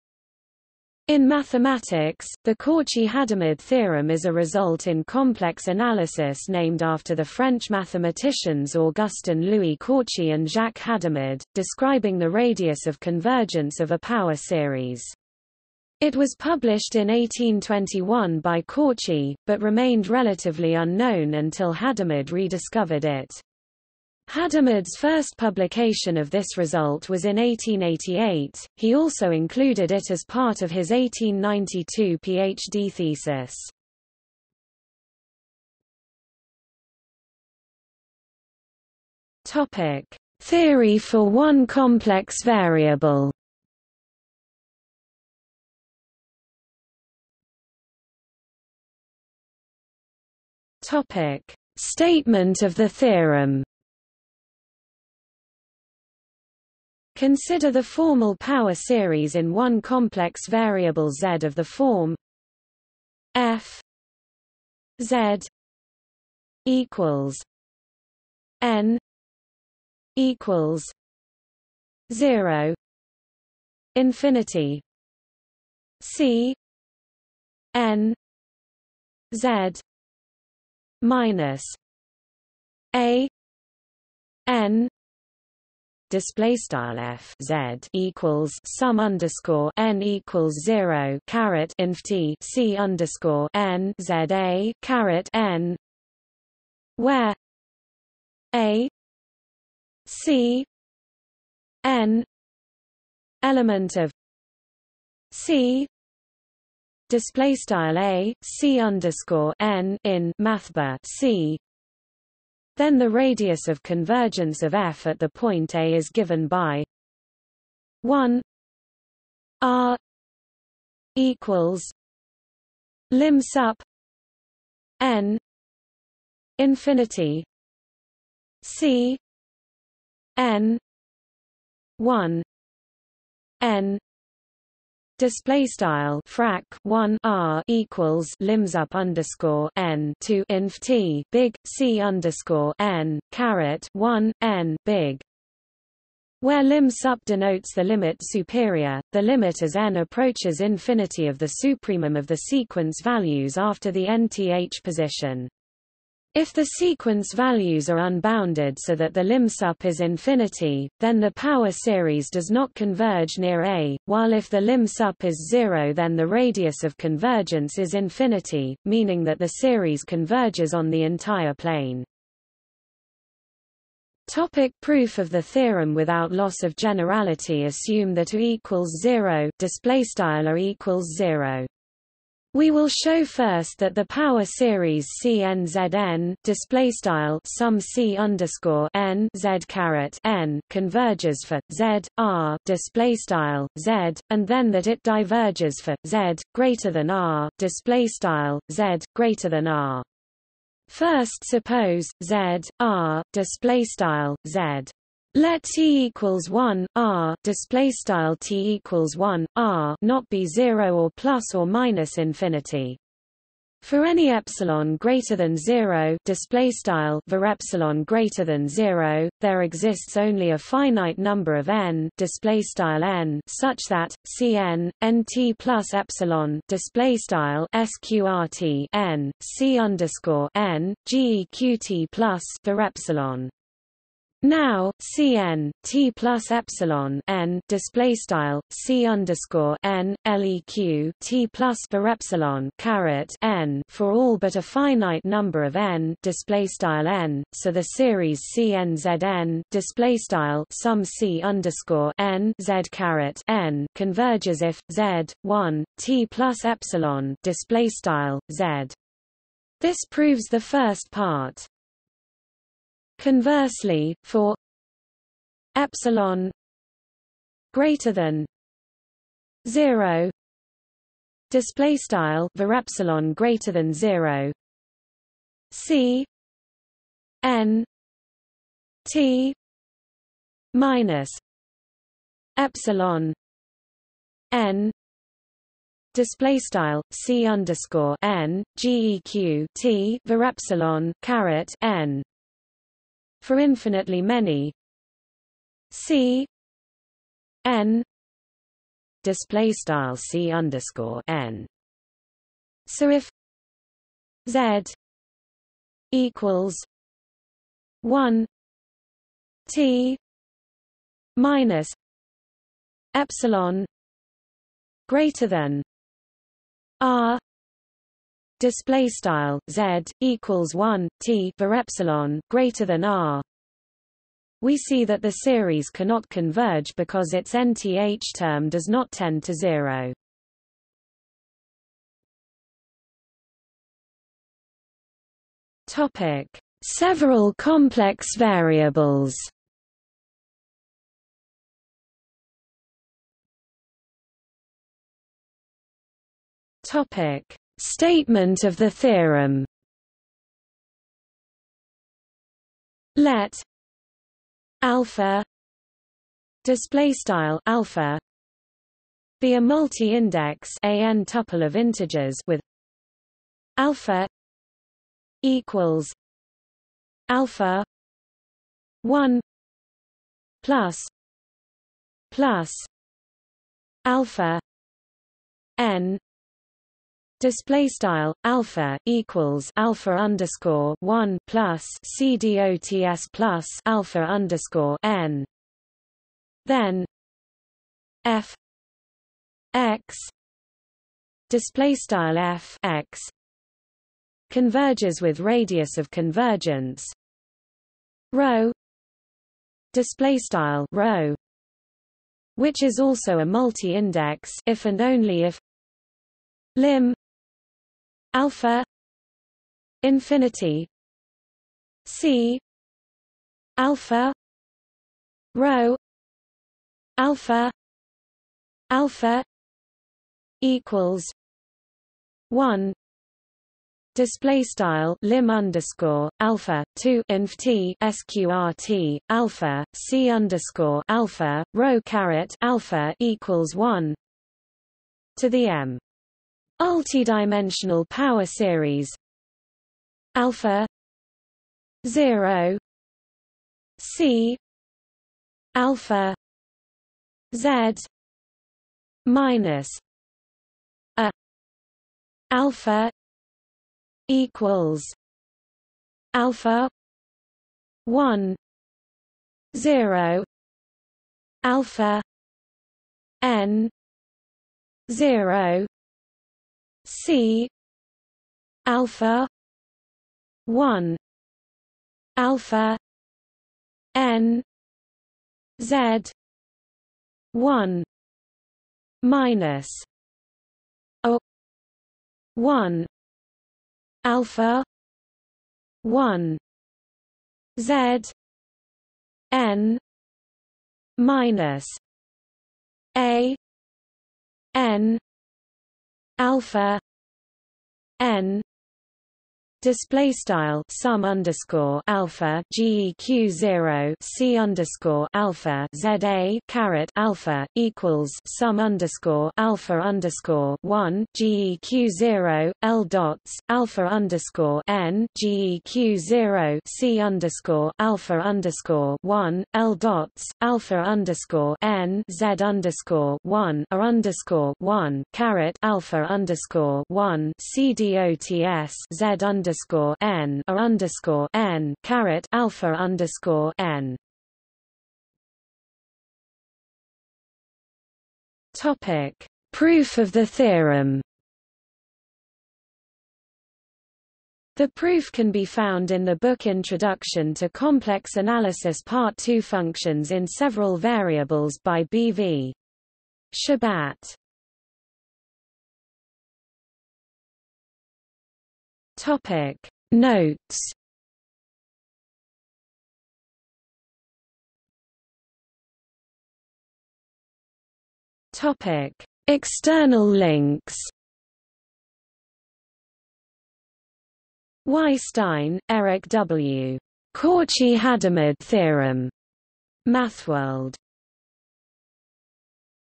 Cauchy–Hadamard theorem | Wikipedia audio article
00:00:35 1 Theory for one complex variable
00:00:47 1.1 Statement of the theorem
00:02:54 1.2 Proof of the theorem
00:05:32 2 Several complex variables
00:05:43 2.1 Statement of the theorem
00:08:55 2.2 Proof of the theorem
00:09:15 3 Notes
00:09:24 4 External links

- Socrates

SUMMARY
In mathematics, the Cauchy–Hadamard theorem is a result in complex analysis named after the French mathematicians Augustin Louis Cauchy and Jacques Hadamard, describing the radius of convergence of a power series. It was published in 1821 by Cauchy, but remained relatively unknown until Hadamard rediscovered it. Hadamard's first publication of this result was in 1888; he also included it as part of his 1892 Ph.D. thesis.